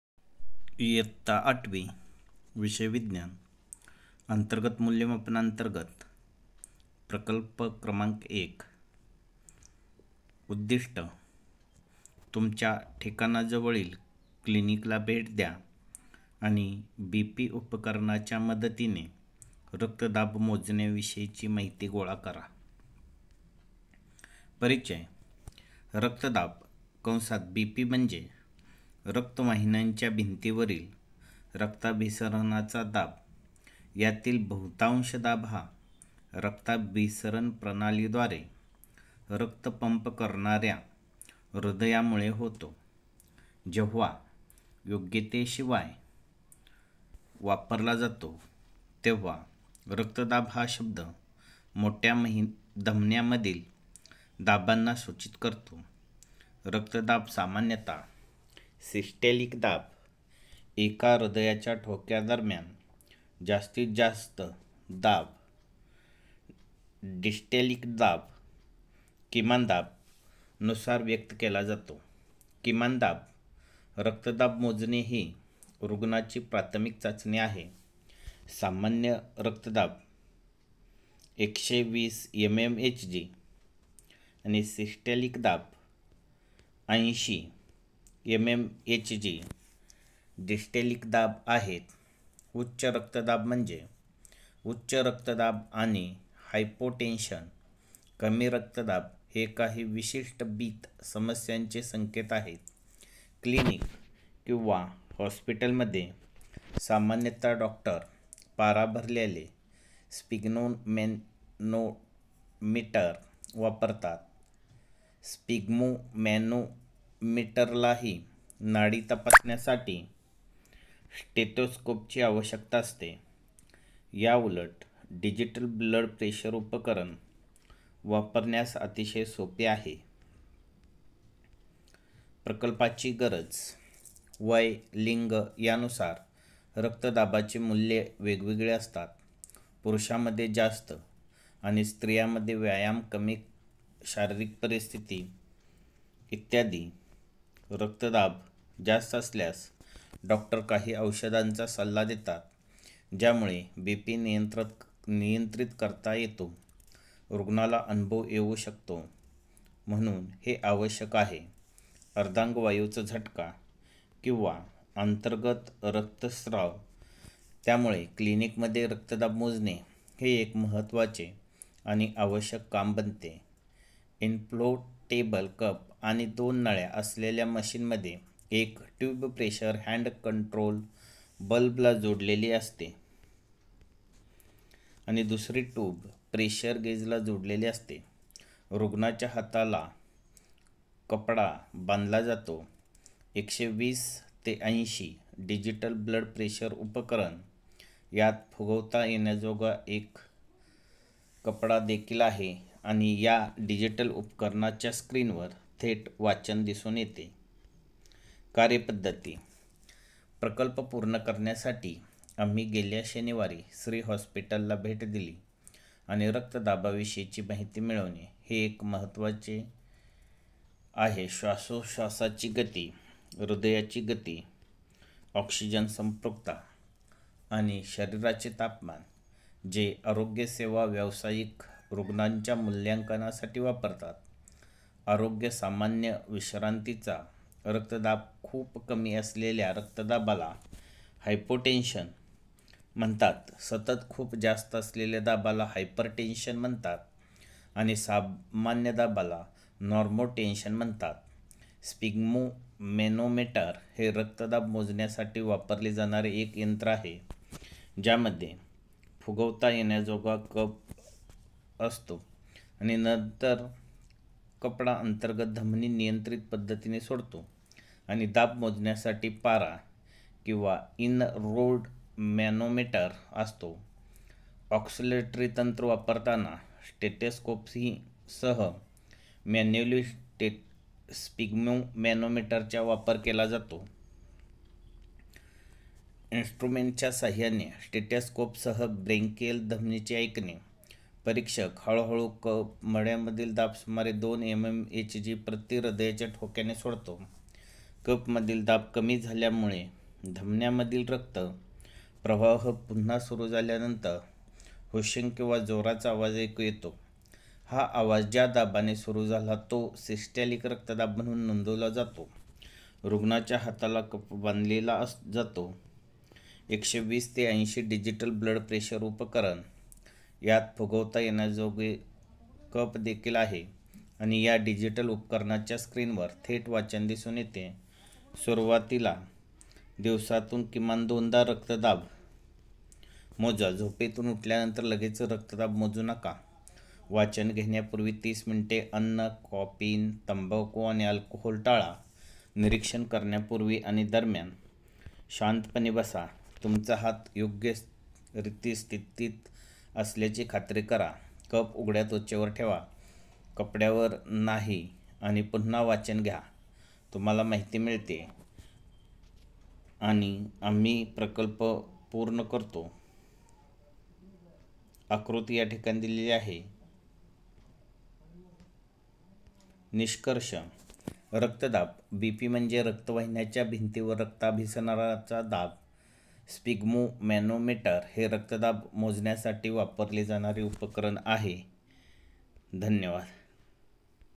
इयत्ता 8 वी विज्ञान प्रकल्प क्र 1#बीपी उपकरणाच्या मदतीने रक्तदाब मोजणे माहिती गोळा करा.prakalp
उद्दिष्ट: तुमच्या ठिकाणाजवळील क्लिनिकला भेट द्या आणि बीपी उपकरणाच्या मदतीने रक्तदाब मोजण्याविषयी माहिती गोळा करा.

परिचय :- रक्तदाब (BP) म्हणजे रक्तवाहिन्यांच्या भिंतींवरील रक्ताभिसरणाचा दाब. यातील बहुतांश दाब हा रक्ताभिसरण प्रणालीद्वारे रक्त पंप करणाऱ्या हृदयामुळे होतो. जेव्हा योग्यतेशिवाय वापरला जातो तेव्हा "रक्तदाब" हा शब्द मोठ्या धमन्यांमधील दाबांना सूचित करतो. रक्तदाब सामान्यतः सिस्टोलिक दाब (एका हृदयाच्या ठोक्यादरम्यान जास्तीत जास्त दाब) डायस्टोलिक दाब (किमान दाब) नुसार व्यक्त केला जातो (किमान दाब) रक्तदाब मोजणे ही रुग्णाची प्राथमिक चाचणी आहे. सामान्य रक्तदाब मूल्ये 120 mmHg (सिस्टोलिक दाब) आणि 80 mmHg (डायस्टोलिक दाब) आहेत. उच्च रक्तदाब म्हणजे उच्च रक्तदाब आणि हायपोटेन्शन . कमी रक्तदाब हे काही विशिष्ट बीथ समस्यांचे संकेत आहेत. क्लिनिक किंवा हॉस्पिटलमध्ये सामान्यतः डॉक्टर पारा भरलेले स्फिग्मोमॅनोमीटर वापरतात.
स्फिग्मोमॅनोमीटरलाही नाडी तपासण्यासाठी स्टेथोस्कोपची आवश्यकता असते. याउलट डिजिटल ब्लड प्रेशर उपकरण वापरण्यास अतिशय सोपे आहे.

प्रकल्पाची गरज :-

वय, लिंग यानुसार रक्तदाबाची मूल्ये वेगवेगळी असतात (पुरुषांमध्ये जास्त आणि स्त्रियांमध्ये व्यायाम कमी. शारीरिक परिस्थिती इ. रक्तदाब जास्त असल्यास, डॉक्टर काही औषधांचा सल्ला देतात ज्यामुळे बीपी नियंत्रित करता येतो. रुग्णाला अनुभव येऊ शकतो म्हणून हे आवश्यक आहे. अर्धांगवायूचा झटका किंवा अंतर्गत रक्तस्त्राव. त्यामुळे, क्लिनिकमध्ये रक्तदाब मोजणे हे एक महत्त्वाचे आणि आवश्यक काम बनते. इन्फ्लाटेबल कफ आणि दोन नळ्या असलेल्या मशीनमध्ये: एक ट्यूब प्रेशर हँड कंट्रोल बल्बला जोडलेली असते आणि दुसरी ट्यूब प्रेशर गेजला जोडलेली असते. रुग्णाच्या हाताला कफ बांधला जातो.
120- 80
डिजिटल ब्लड प्रेशर उपकरण: यात फुगवता येण्याजोगा कफ देखील आहे आणि या डिजिटल उपकरणाच्या स्क्रीनवर थेट वाचन दिसून येते.

कार्यपद्धती:-प्रकल्प पूर्ण करण्यासाठी आम्ही गेल्या शनिवारी *श्री हॉस्पिटल* ला भेट दिली आणि रक्तदाबाविषयी माहिती मिळवणे ही एक महत्त्वाची चिन्हे आहे—श्वासोच्छ्वासाची गती, हृदय गती, ऑक्सिजन संपृक्तता आणि शरीराचे तापमान—जे आरोग्यसेवा व्यावसायिक रुग्णाच्या मूल्यांकनासाठी वापरतात. आरोग्य सामान्य विश्रांतीचा रक्तदाब
खूप कमी असलेल्या रक्तदाबाला हायपोटेन्शन म्हणतात, सतत खूप जास्त असलेल्या दाबाला हायपरटेन्शन म्हणतात आणि सामान्य दाबाला नॉर्मोटेन्शन म्हणतात.
स्फिग्मोमॅनोमीटर हे रक्तदाब मोजण्यासाठी वापरले जाणारे एक यंत्र आहे, ज्यामध्ये फुगवता येण्याजोगा कफ कोसळतो आणि नंतर कफ अंतर्गत धमनी नियंत्रित पद्धतीने सोडतो आणि दाब मोजण्यासाठी पारा किंवा एनरोइड मॅनोमीटर असतो. ऑस्कल्टरी तंत्र वापरताना स्टेथोस्कोपसह मॅन्युअल स्फिग्मोमॅनोमीटरचा वापर केला जातो. मॅन्युअल इन्स्ट्रुमेंटच्या सहाय्याने, स्टेथोस्कोपसह ब्रॅचियल धमनीचे ऐकणे, परीक्षक हळू हळू कफमधील दाब सुमारे 2 mmHg प्रति हृदयाच्या ठोक्याने सोडतो. कफमधील दाब कमी झाल्यामुळे, धमनीमध्ये रक्त प्रवाह पुन्हा सुरू झाल्यावर "हुशिंग" किंवा जोराचा आवाज ऐकू येतो. हा आवाज ज्या दाबाने सुरू झाला तो सिस्टोलिक रक्तदाब म्हणून नोंदवला जातो
रुग्णाच्या हाताला कफ बांधला जातो.
120- 80डिजिटल ब्लड प्रेशर उपकरण: यात फुगवता येण्याजोगा कफ देखील आहे आणि या डिजिटल उपकरणाच्या स्क्रीनवर थेट वाचन दिसून येते.सुरुवातीला, दिवसातून किमान दोनदा रक्तदाब मोजा झोपेतून उठल्यानंतर लगेच रक्तदाब मोजू नका.वाचन घेण्यापूर्वी 30 मिनिटे अन्न, कॅफीन, तंबाखू आणि अल्कोहोल टाळा निरीक्षण करण्यापूर्वी आणि दरम्यान शांतपणे बसा.
तुमचा हात योग्यरित्या स्थित असल्याची खात्री करा.कफ उघड्या त्वचेवर ठेवा, कपड्यांवर नाही.
आणि पुन्हा वाचन घ्या. आम्हाला माहिती मिळते आणि आम्ही प्रकल्प पूर्ण करतो.

निष्कर्ष :-रक्तदाब (BP) म्हणजे रक्तवाहिन्यांच्या भिंतींवर रक्ताभिसरणाचा दाब. स्फिग्मोमॅनोमीटर हे रक्तदाब मोजण्यासाठी वापरले जाणारे उपकरण आहे.